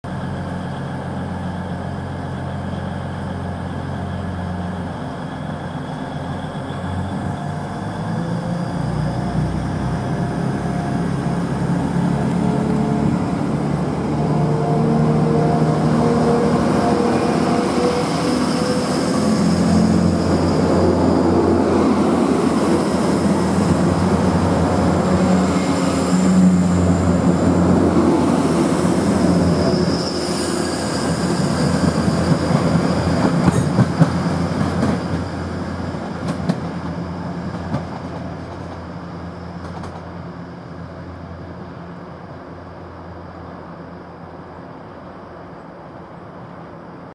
First Hull Trains Class 180 Departing Retford (19/1/15)
Class 180 First Hull Trains 5 Coach Service Departing Retford For Hull. Calling At : Doncaster , Selby , Howden , Brough And Hull. Certainly Hear This Depart With The Mighty DMU Engines.